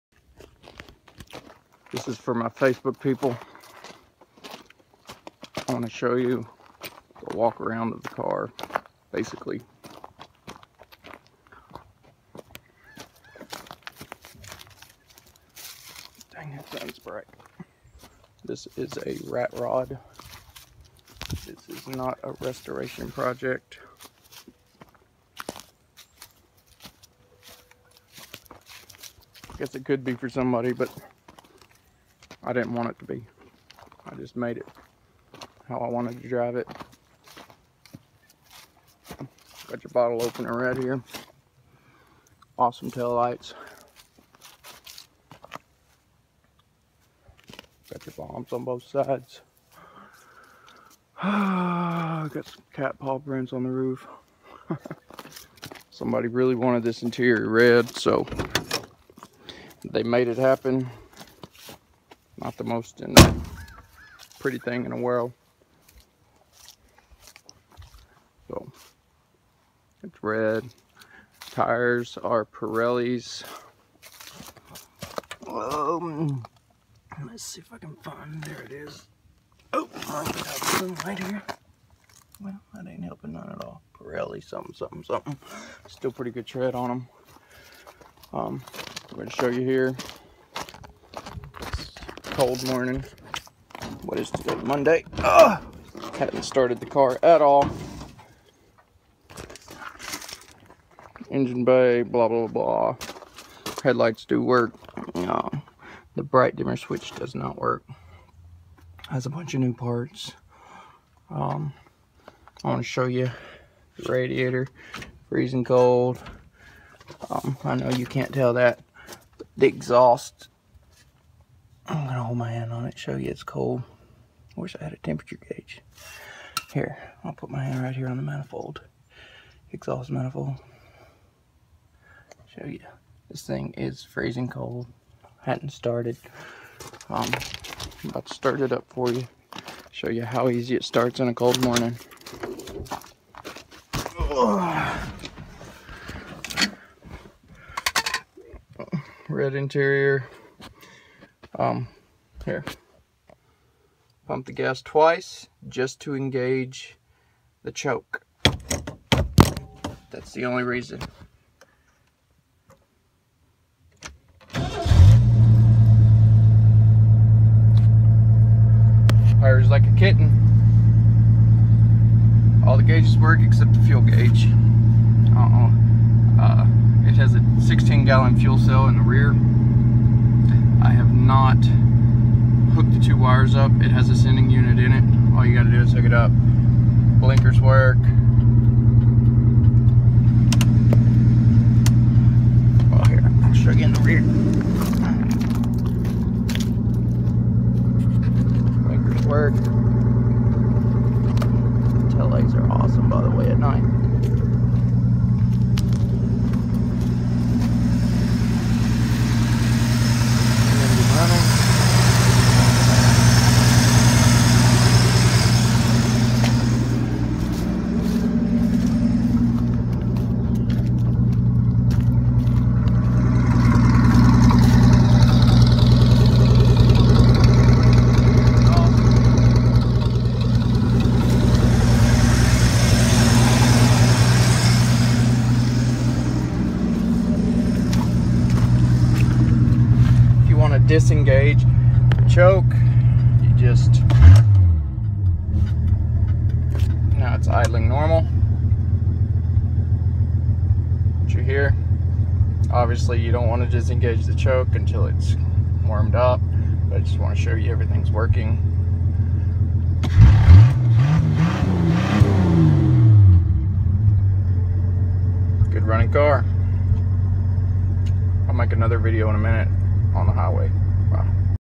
1966 galaxie rat rod
I got this thing going after sitting for 30 years dropped it painted it to look like rust and ww2 bomber plane. Just to sell it. It is sold